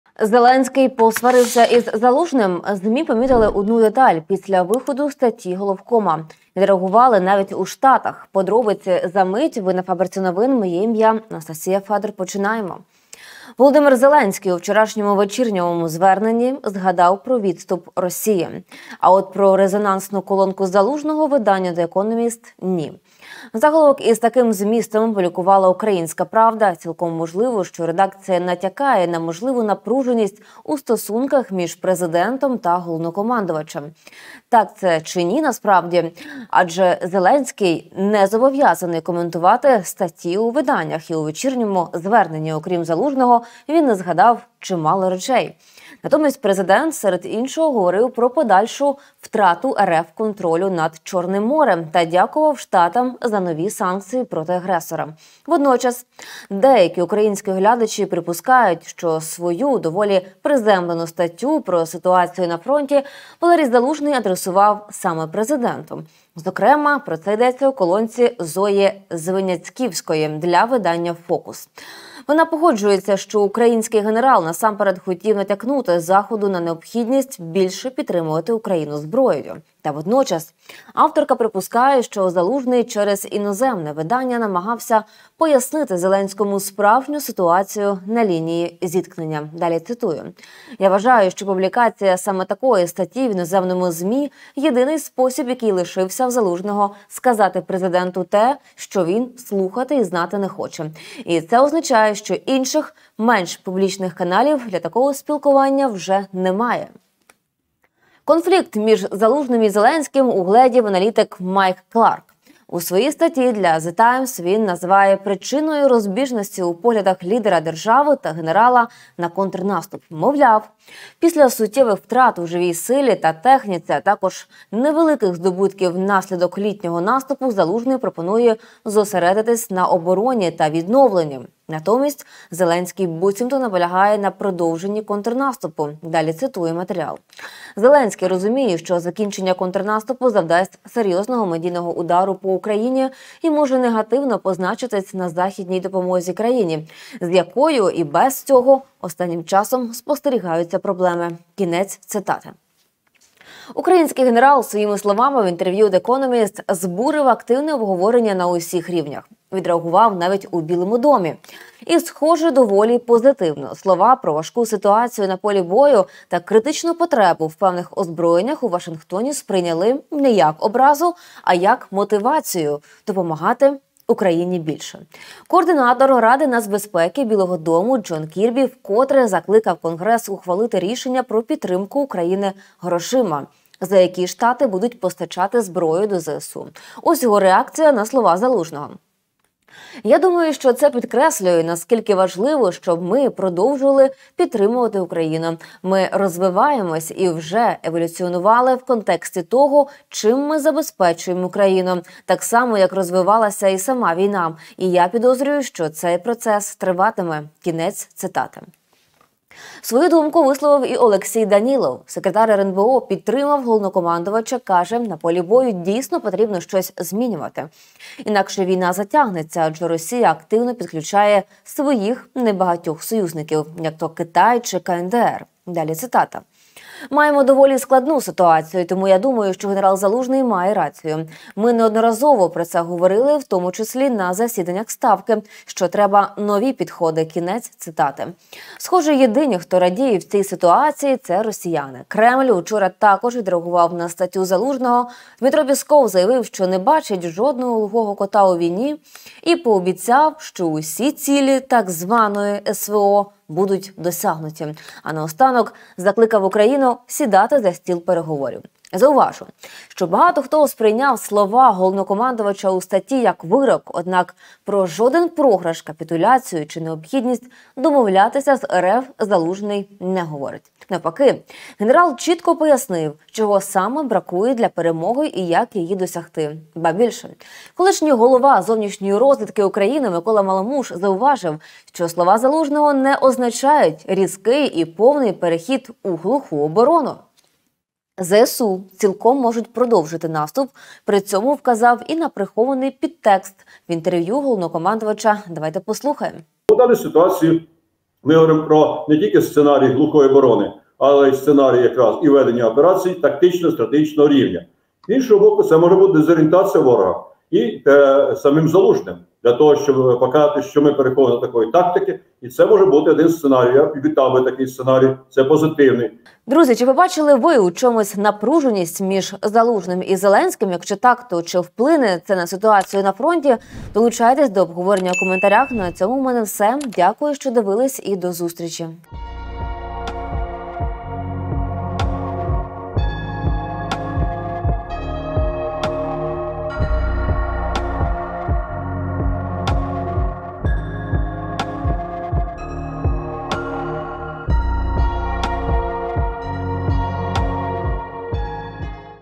💥Побачили СВАРКУ ЗАЛУЖНОГО І ЗЕЛЕНСЬКОГО! Усе через наступ ЗСУ. Останні заяви - ляпас президенту?
Володимир Зеленський у вчорашньому вечірньому зверненні згадав про відступ Росії. А от про резонансну колонку Залужного виданню The Economist – ні. Такий матеріал опублікувала Українська правда. Цілком можливо, що редакція натякає на можливу напруженість у стосунках між Президентом та Головнокомандувачем. Деякі українські оглядачі припускають, що свою статтю про ситуацію на фронті Валерій Залужний адресував саме Президенту. Зокрема, про це йдеться у колонці Зої Звиняцківської для видання “Фокус”. Авторка припускає, що Залужний через іноземне видання намагався пояснити Зеленському справжню ситуацію на лінії зіткнення. Деталі - на “Фабриці новин”.